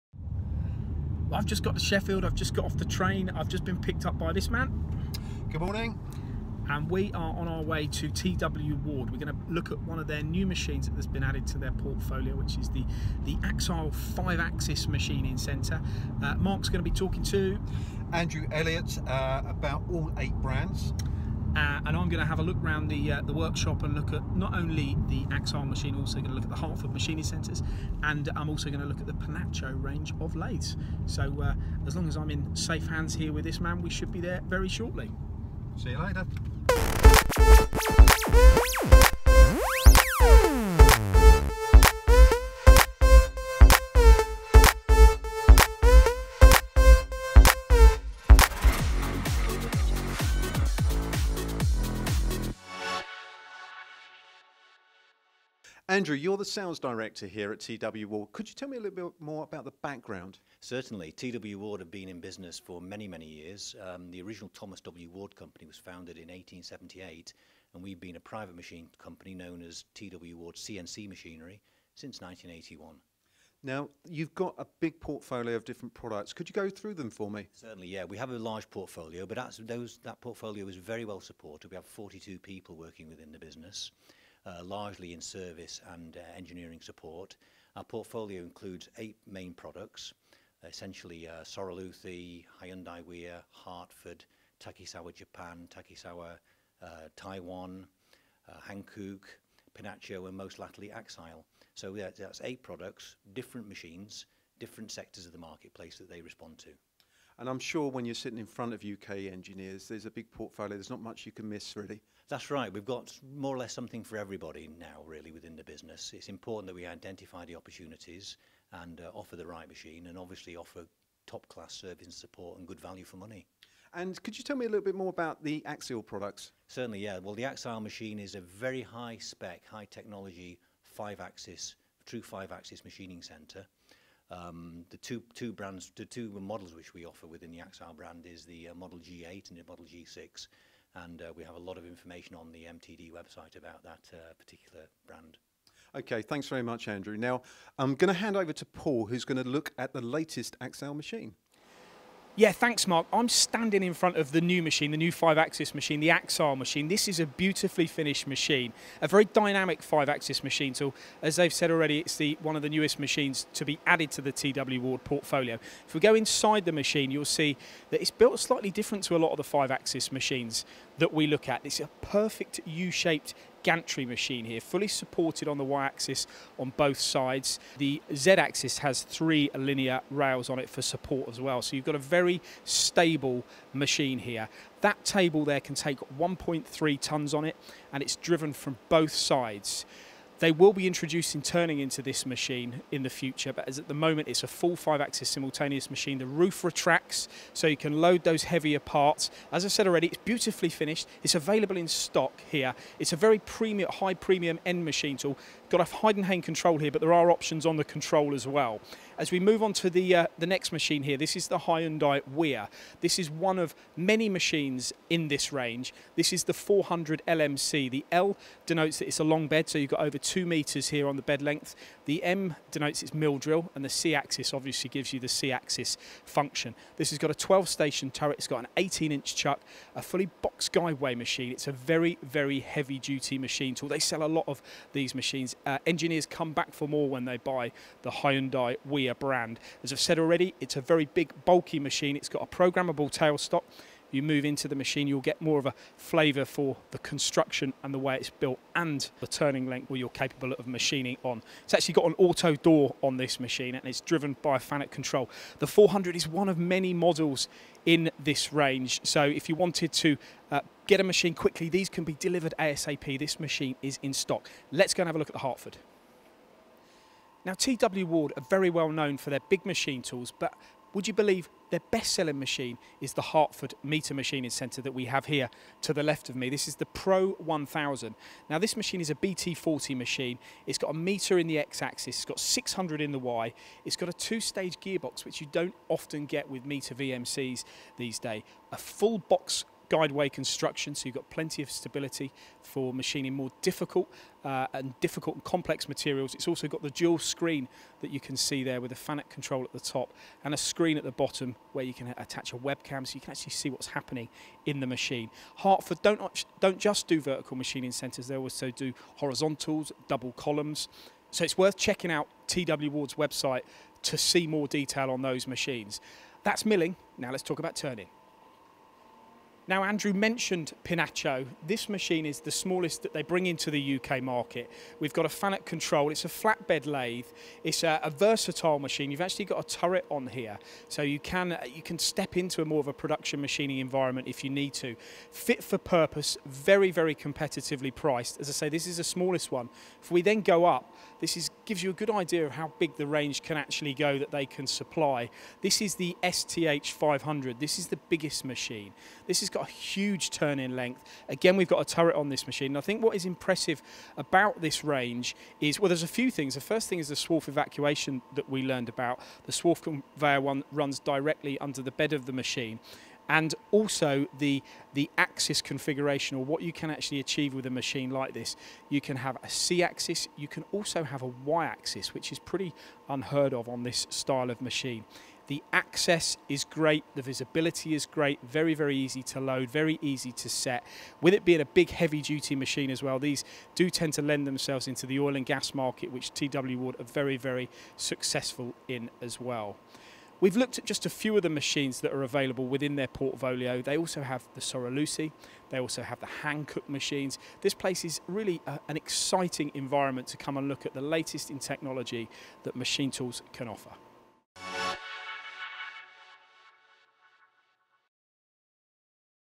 Welcome to Ward CNC in Sheffield
MTDCNC takes a look around the impressive premises of Ward CNC in Sheffield. Ward CNC offer a wide range of machines from both their Sheffield and Redditch facilities, with the emphasis being on reliability, both of machines and support. A great range of machines for a wide variety of industry needs under one roof. To find out more about Ward CNC and what they can offer you their customer, visit our website you may be surprised at what you find or give us a call to view our showrooms.